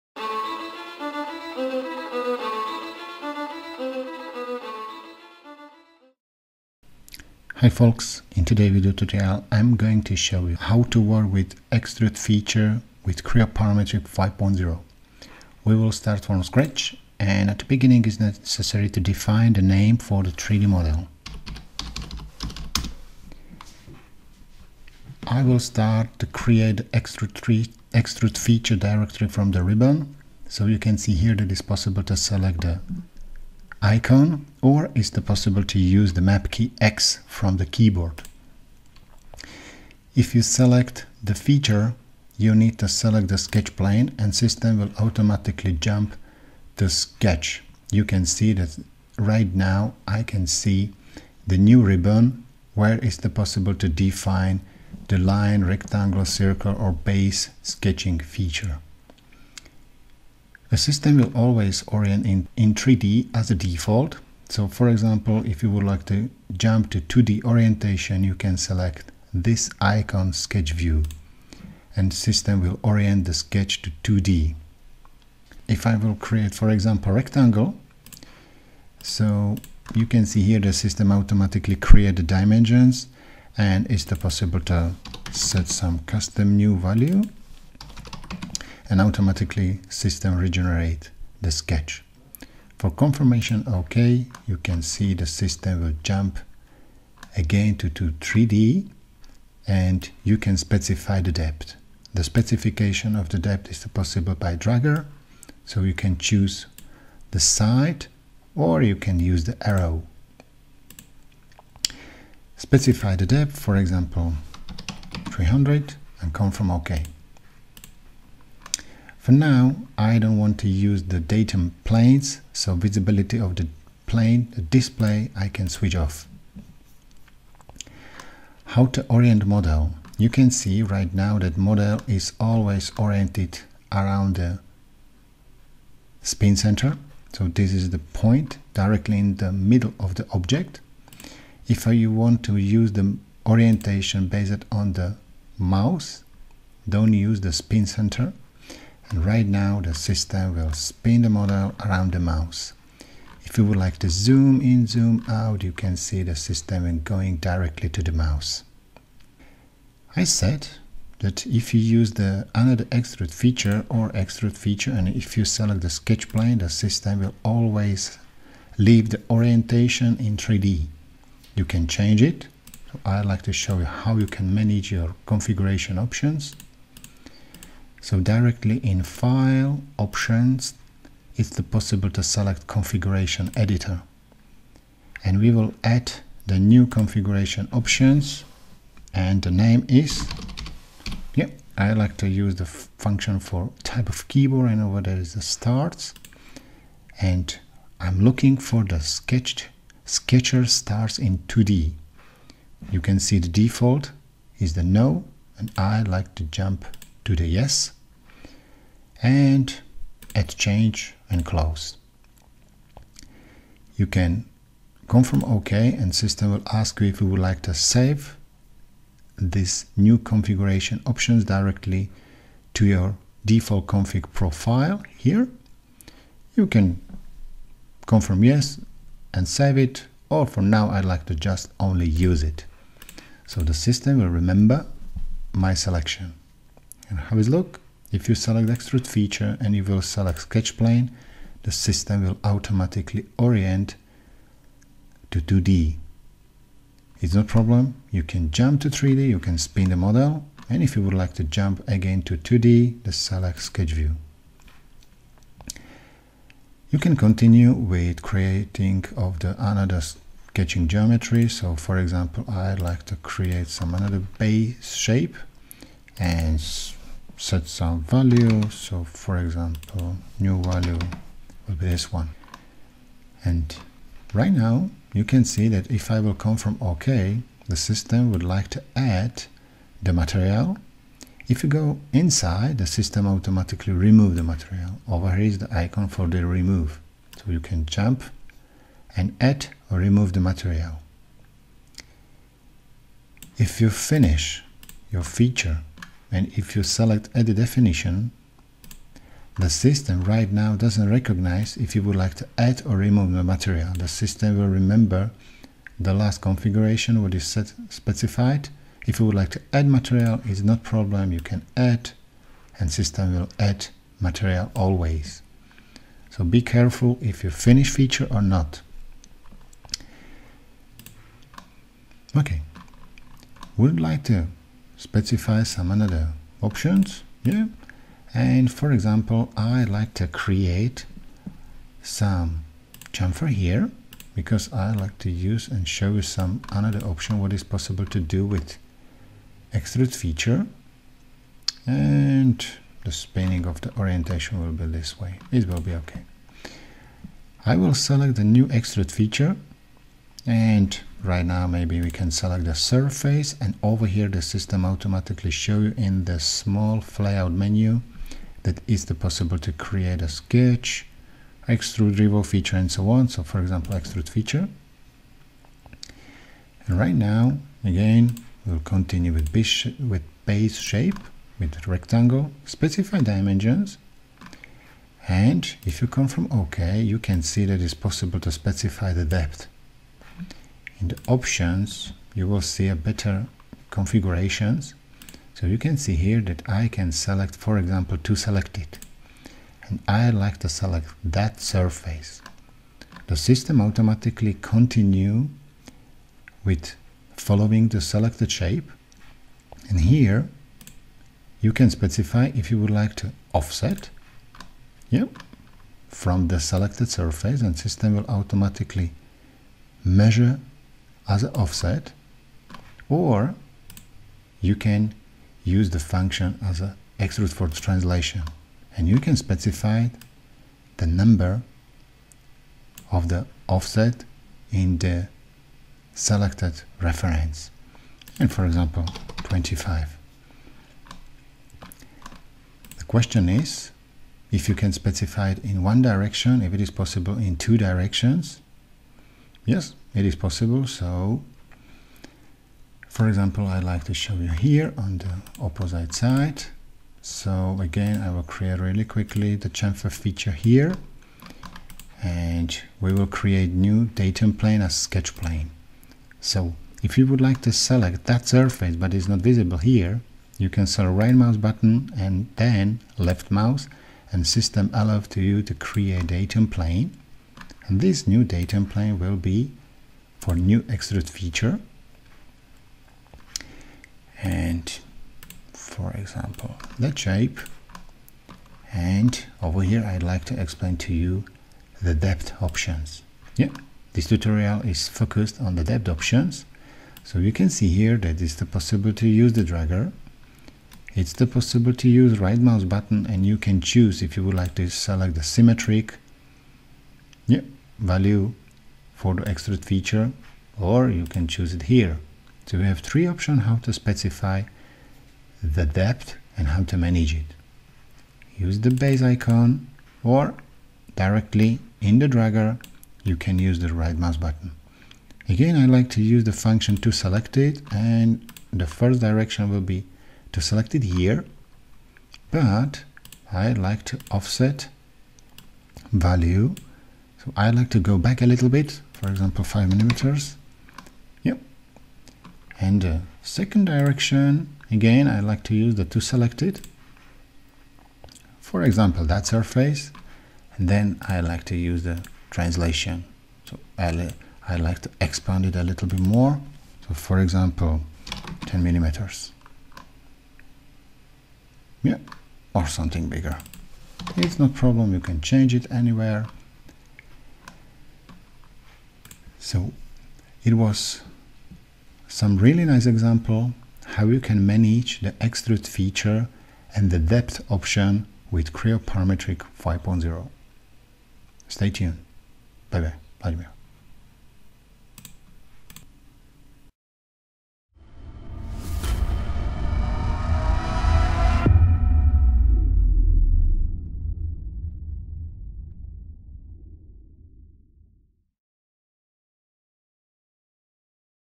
What's New In Creo 5.0 - How to manage Depth for Extrude feature
With Extrude feature is possible to define the couple of depth options and here is the tutorial for you - How to manage Depth for Extrude feature

This video was created based on question from a comment under my video on YouTube channel.

If you enjoy my content, please consider supporting what I do: Buy a Coffee for 4KSide